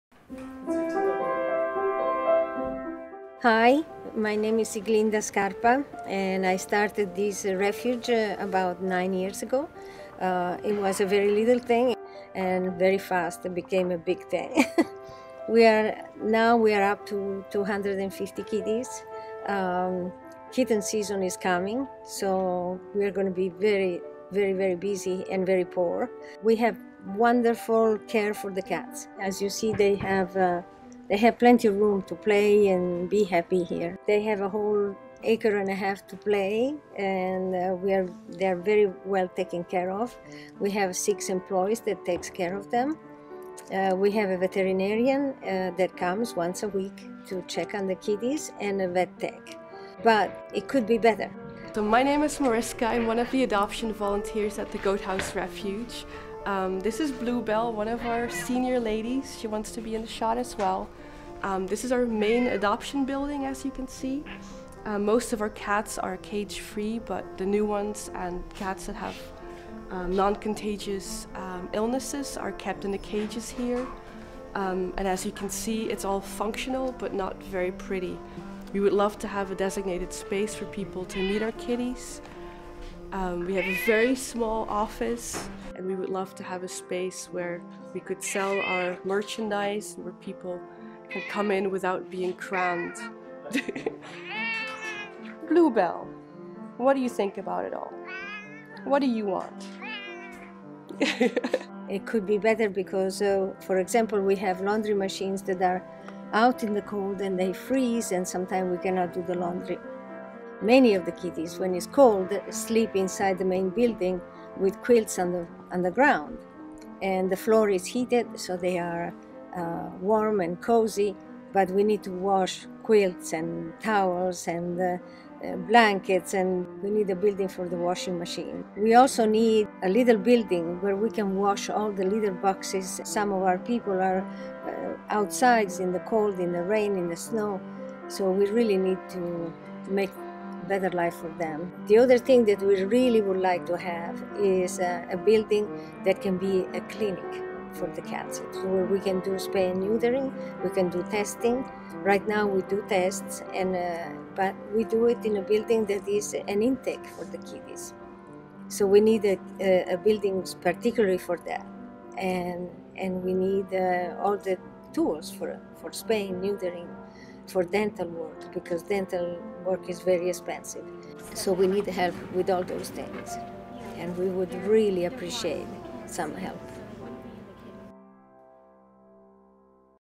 Pet Nation Renovation Goathouse Refuge2
Goathouse Refuge is a no-kill cat sanctuary in Pittsboro, NC that homes about 300 cats. We take in cats from kill shelters throughout North Carolina and Southeastern U.S., and also other unwanted cats, regardless of their age or medical conditions. Last year alone, we adopted out 367 cats.